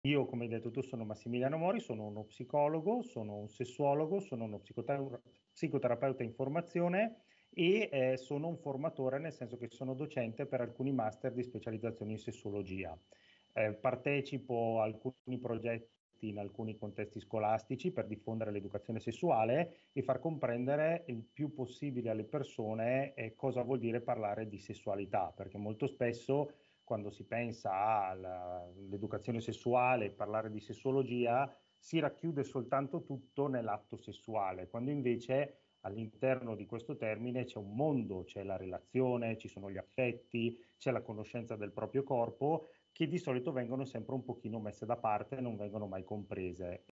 Chi è Massimiliano Mori
Ecco una breve presentazione di Massimiliano Mori, ospite dell'ultimo video sul canale
Grafiche create da:

 / jajaja.studio